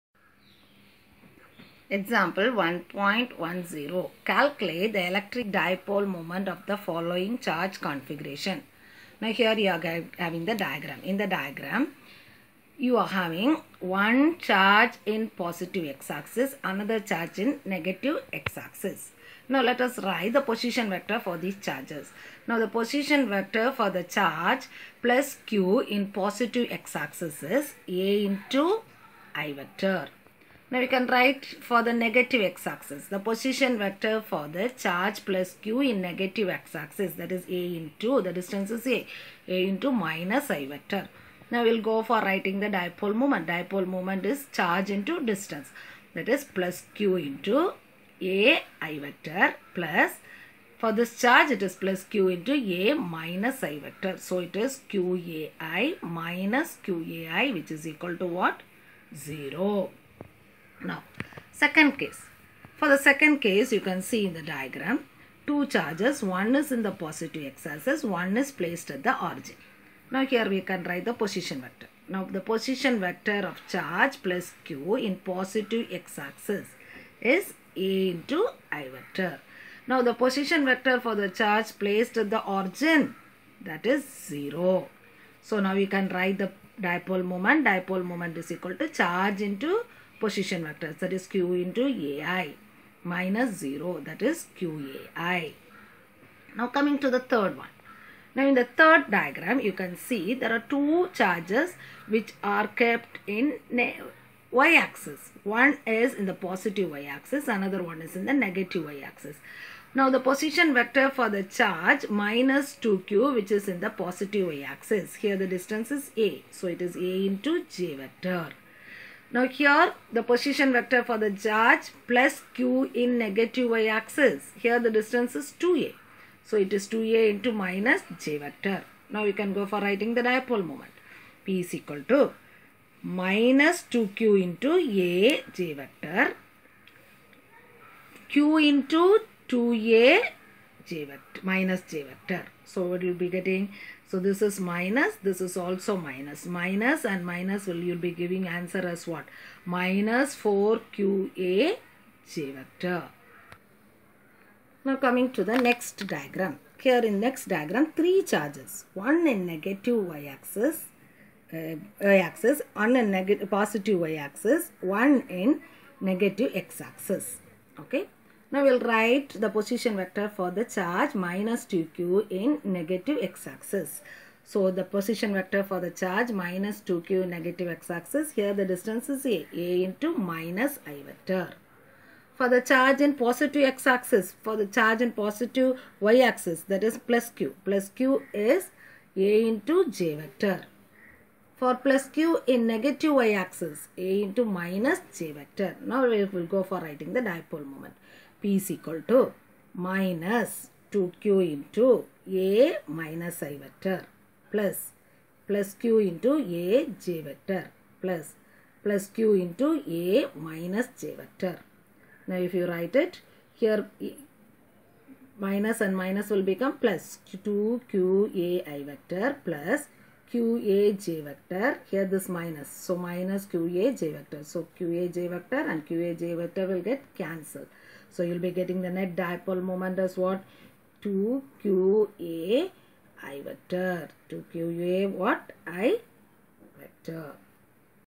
STANDARD Xll -ELECTROSTATICS -EXAMPLE 1.10
Calculate the electric dipole moment for the following charge configurations.